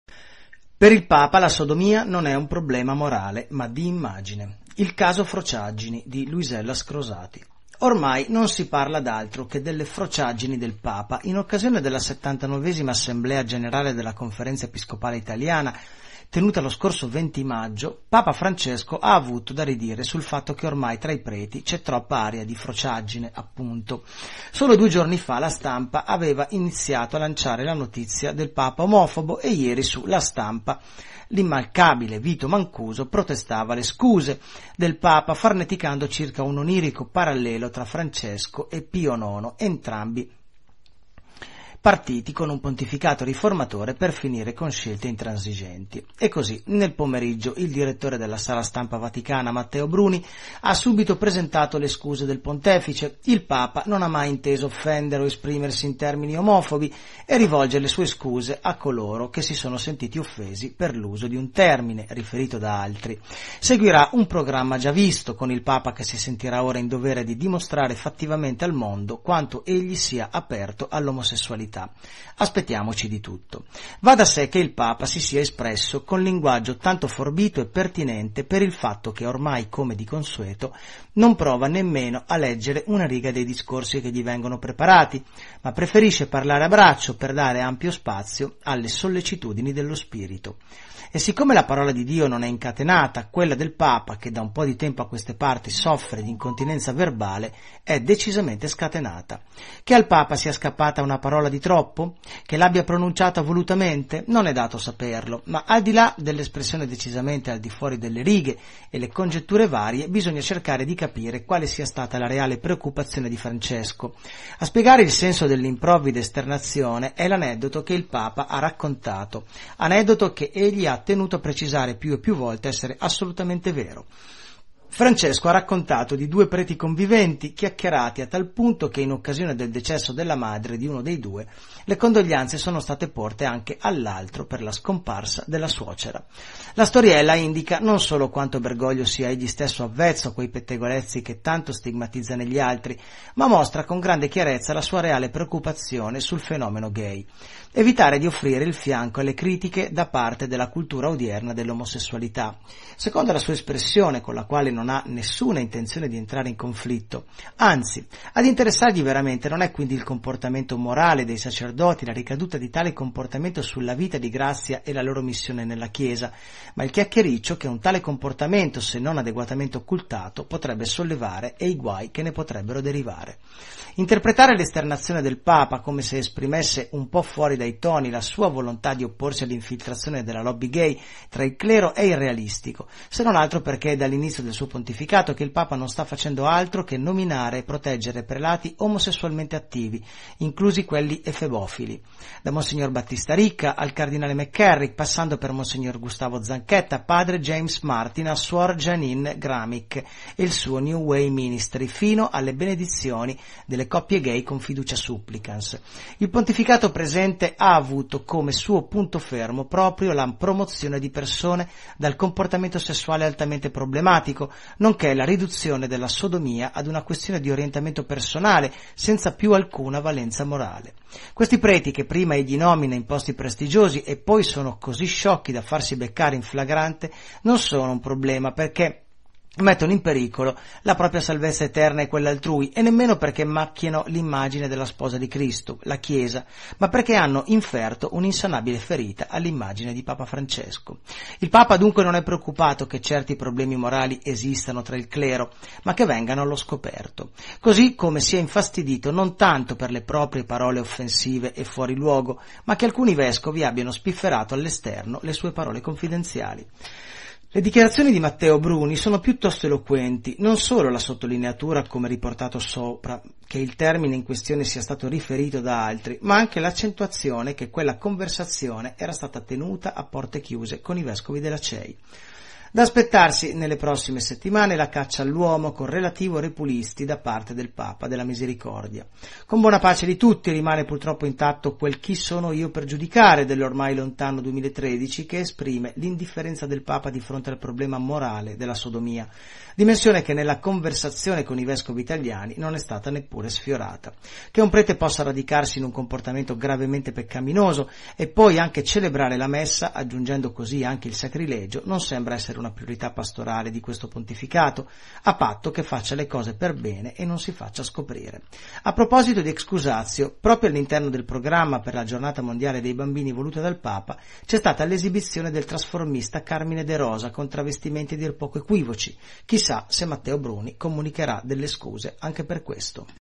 Per il Papa la sodomia non è un problema morale, ma di immagine
Le "frociaggini" di Francesco vanno inquadrate nel discorso ai vescovi, non come una lotta alla lobby gay vaticana: ad interessargli non è il comportamento morale dei sacerdoti, ma il chiacchiericcio che questo potrebbe sollevare. Infatti dall'inizio del suo pontificato non ha fatto altro che proteggere prelati omosessuali attivi.